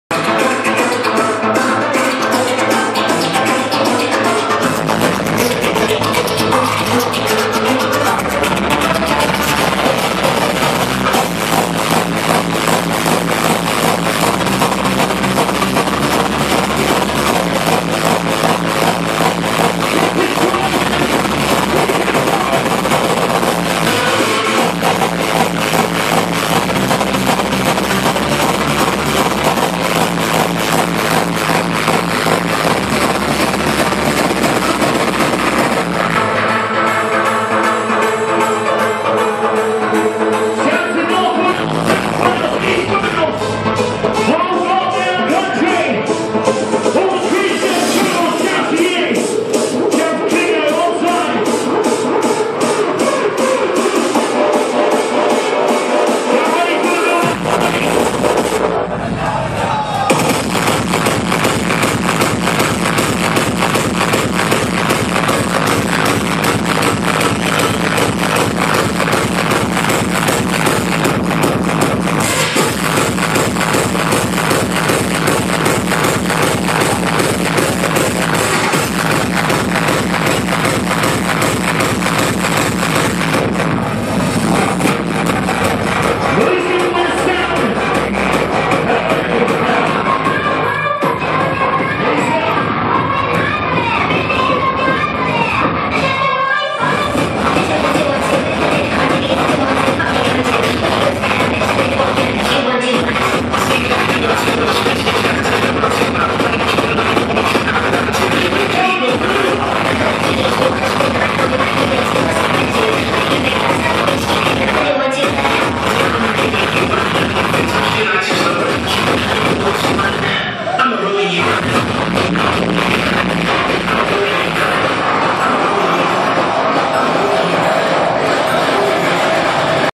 BRIAN M VS MCBUNNS Keep it Hardstyle Show set! video (5) HD
From Bionic's 12th Birthday at Cardiff Uni on 14th July 2012! enjoy!!!:)